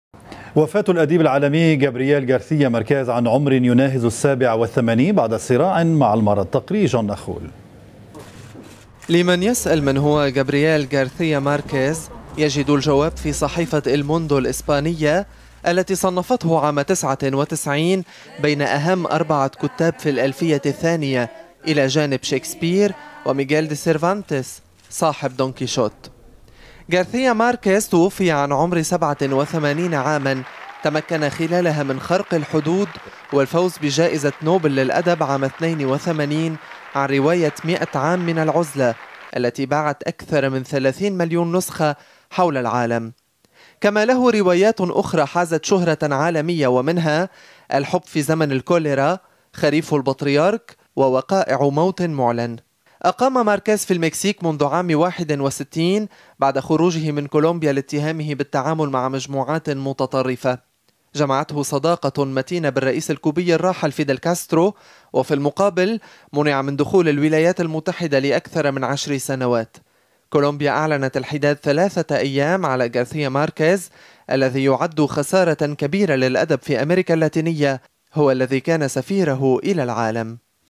Gabrielle Garcia Marquez Death - MTV Lebanon - Jean Nakhoul - 18-04-2014
وفاة الأديب العالمي غابريال غارسيا ماركيز.
تقرير جان نخول.